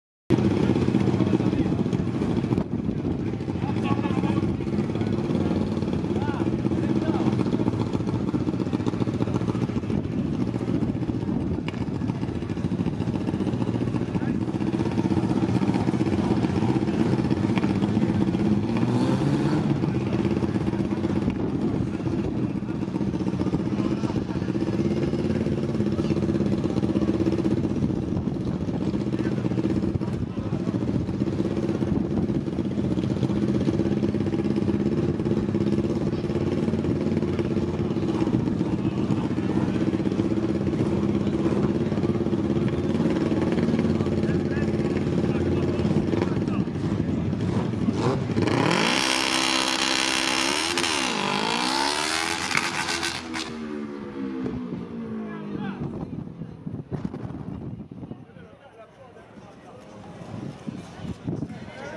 October 2023 RWYB Run 32
23rd October 2023

Test and Tune day, also known as a Run What You Bring, held 2 weeks before the upcoming October Quarter mile event.